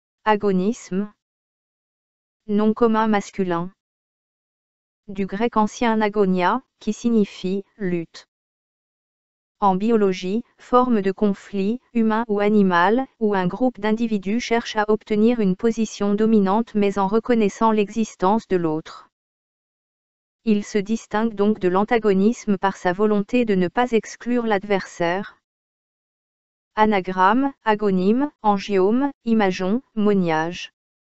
Agonisme | Dictionnaire français audio
Agonisme.
Nom commun masculin.
Du grec ancien agônia, qui signifie « lutte ».
En Biologie, Forme de conflit, humain ou animal, où un groupe d’individus cherche à obtenir une position dominante mais en reconnaissant l’existence de l’autre. Il se distingue donc de l’antagonisme par sa volonté de ne pas exclure l’adversaire.
Anagrammes : agonîmes, angiomes, imageons, moniages.